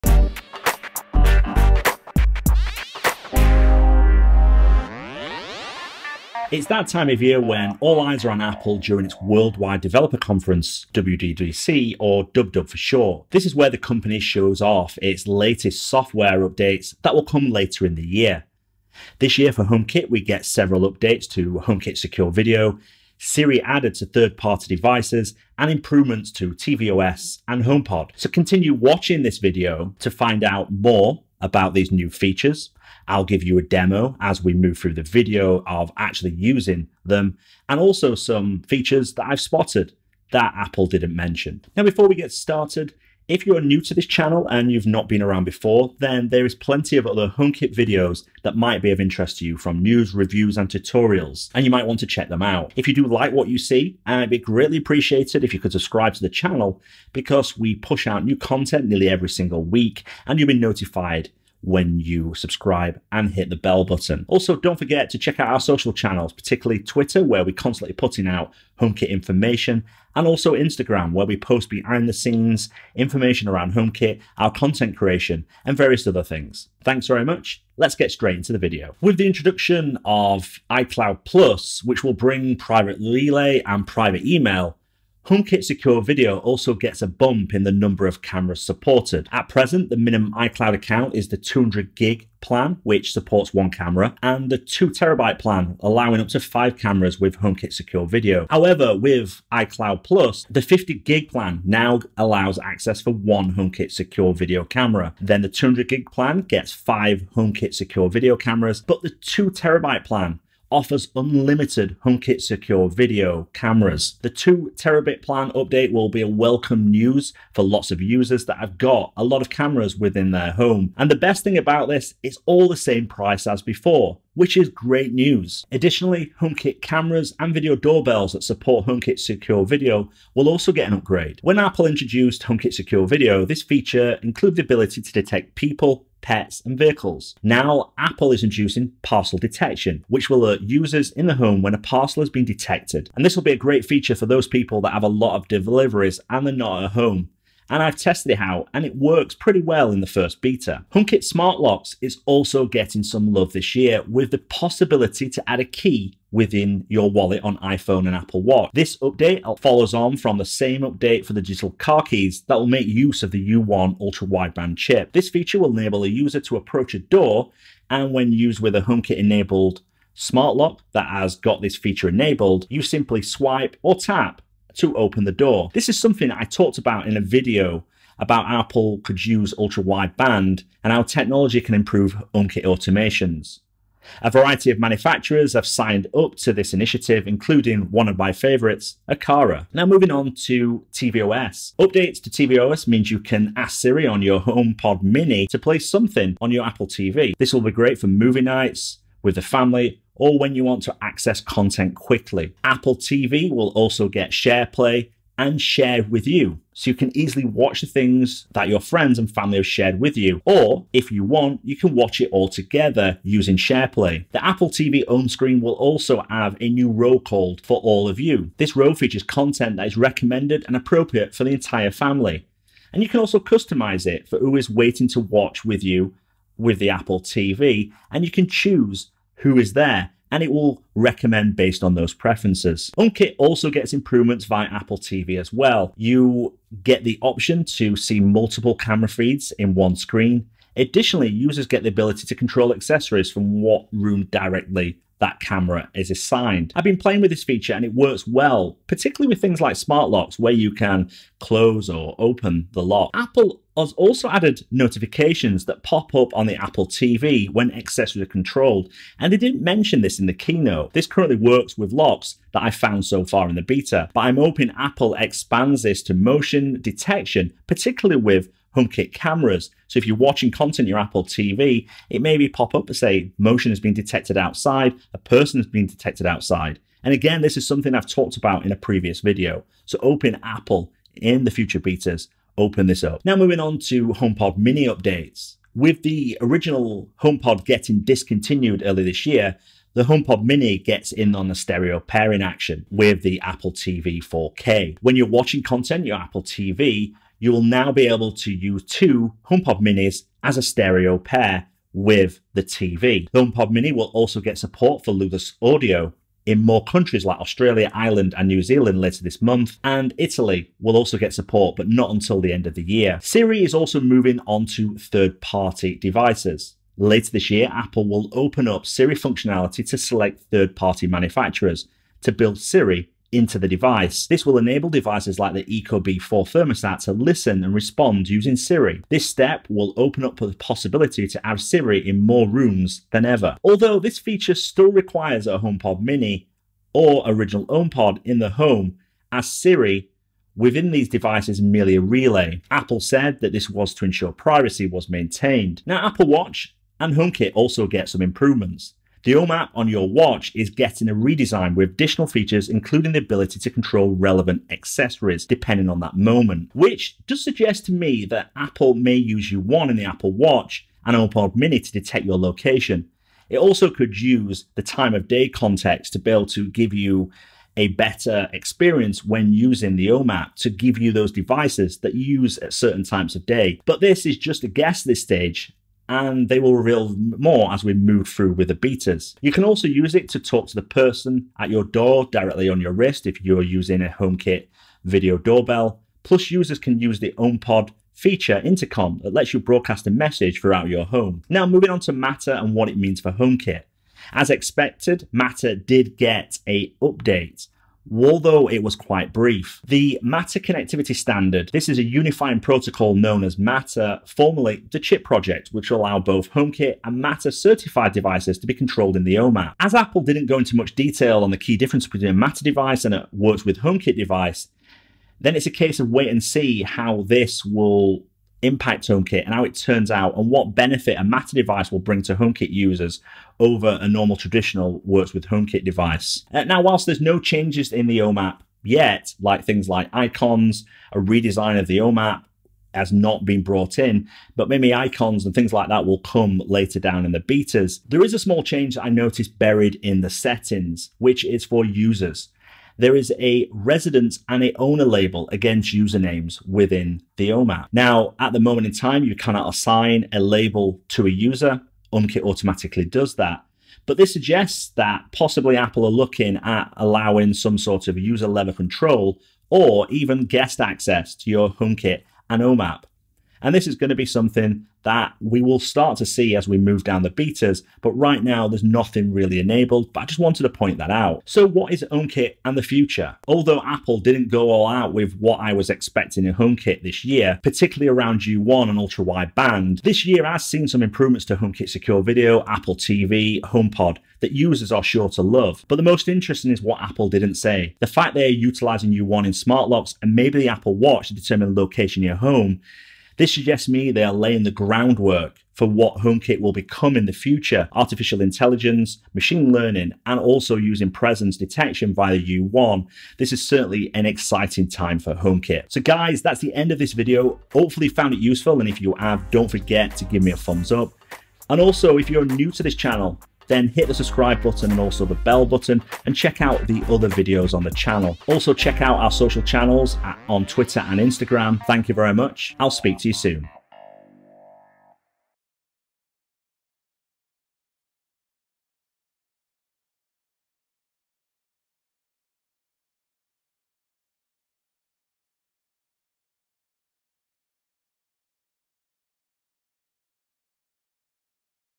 WWDC 21 - Everything new in Apple TV, Apple Home & HomePod for the HomeKit smart home in iOS 15
At a that time when all eyes are on Apple during its Worldwide Developers conference or WWDC for short. This is where the company shows off its latest updates to its device software that will come later in the year.

This year for HomeKit we get several updates to HomeKit Secure Video, Siri added to third party devices and improvements to tvOS and HomePod.
So continue watching to find out all the details from WWDC 2021 and HomeKit and some of the smaller details I have discovered in the first beta's of iOS15, iPadOS 15 and tvOS 15.

Follow me for all the latest on Homekit and smart home reviews

Intro 0:00
Channel intro 0:41
HomeKit Secure Video 1:18
Smart Locks 2:30
tvOS in HomeKit 3:09
HomePod mini 4:42
Siri on Third party devices 5:12
Apple Watch and HomeKit 5:45
Matter and HomeKit 6:31
HomeKit and the future 7:51